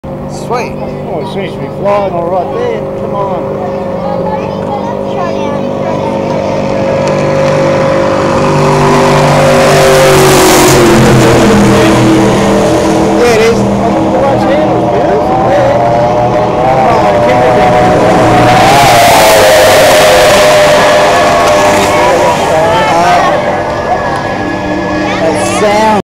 OH THAT SOUND ! WaterSports Marine Triple Rig & Top Shot 2015 Grafton Bridge to Bridge Ski Race
Triple Rigged, Mercury Racing, Bullet Outboard Boat, with Haltech Running Gear & Water Sports Marine Racing know how...
Aussie Innovation and Experimentation, mixed with a whole lotta' fun Ski Racing !